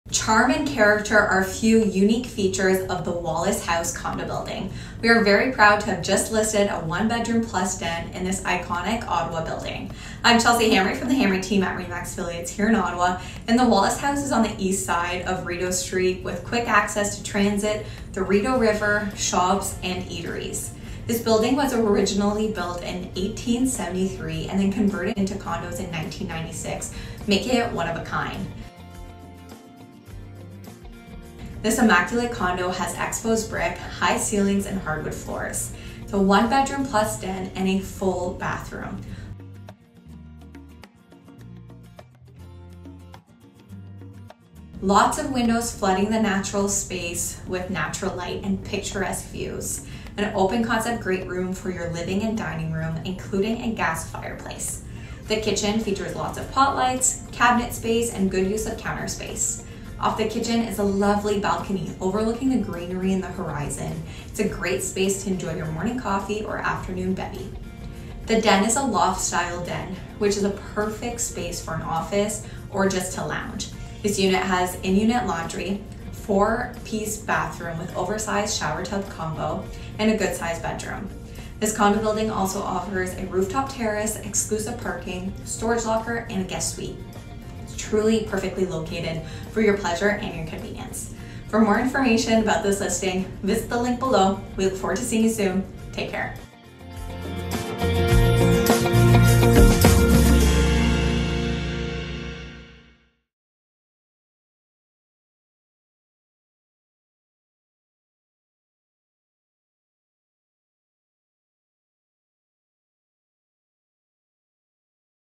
589 Rideau Street #504 - Downtown Ottawa - Hamre Real Estate Team RE/MAX Affiliates Realty Brokerage
🏰 Charm and character are just a few unique features of the Wallis House! ✨

We are proud to have just listed a 1 bedroom + den in this iconic Ottawa condo building. 🏙️

I’m Chelsea Hamre, RE/MAX Affiliates, for the Hamre Team. 🏢

The Wallis House is on the east side of Rideau St, with quick access to transit, the Rideau River, shops, and eateries. 🚇🌊🛍️🍽️

Originally built in 1873 and converted in 1996, this condo is one of a kind. 🏢🏛️

This Immaculate condo has exposed brick, high ceilings, and hardwood floors. 1 bedroom + den, 1 full bath. 🏡🧱🍂🚿

Lots of windows flooding the space with natural light & picturesque views. 🌞🖼️

Open concept great room for your living and dining room, including a gas fireplace. 🛋️🍽️🔥

Kitchen features pot lights, lots of cabinet space, and good use of counter space. 🍳🍽️

Off the kitchen is a lovely balcony overlooking the greenery in the horizon. 🌳🏞️

It’s a great space to enjoy your morning coffee or afternoon bevy. ☕🥂

Den is a loft style, which is perfect for an office or lounge space. In-unit laundry. 🏢💼💼🧺

4 piece bathroom with an oversized shower/tub combo + good sized bedroom. 🚿🛁🛏️

This condo building also offers a rooftop terrace, exclusive parking, storage locker, and a guest suite. 🏙️🌇🚗🗄️🏨

It’s truly Perfectly located for your pleasure and convenience! 📍👉

We look forward to seeing you soon. Take care! 🤗👋